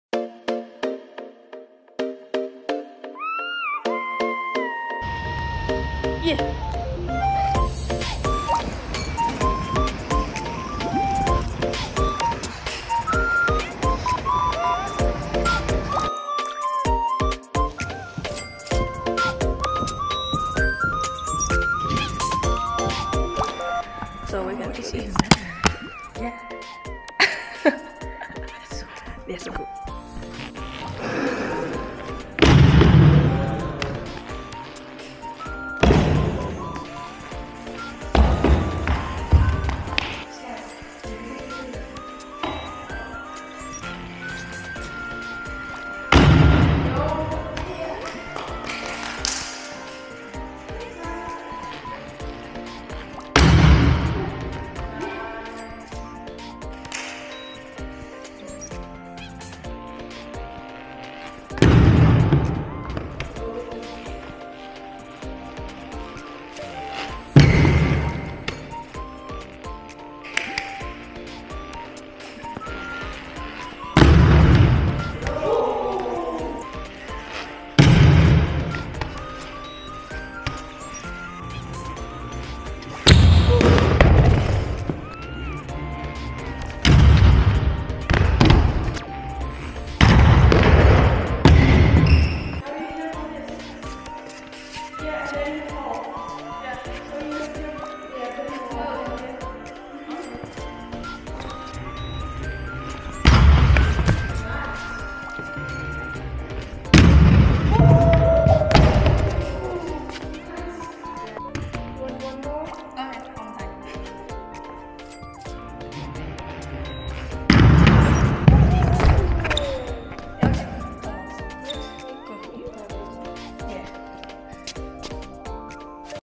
獨輪車Street街式挑戰，首次體驗Street就有大神指導！
荷蘭第四天🇳🇱
第四天終於做正事：騎獨輪車！！
跟偶像見面又一起練車，兩個人包場一大間室內滑板公園，簡直不要太幸福🥺❤️
雖然只有練習1.5小時，但學會了滑短短的平臺，滑斜坡還在努力中，進步神速欸！！
有世界冠軍的加持就是不一樣（？）😍
今天晚上還要再跟她一起去練習，希望能解鎖更多新動作！
下週就要去歐洲極限比賽了(EUC)！！！！好期待🤩🤩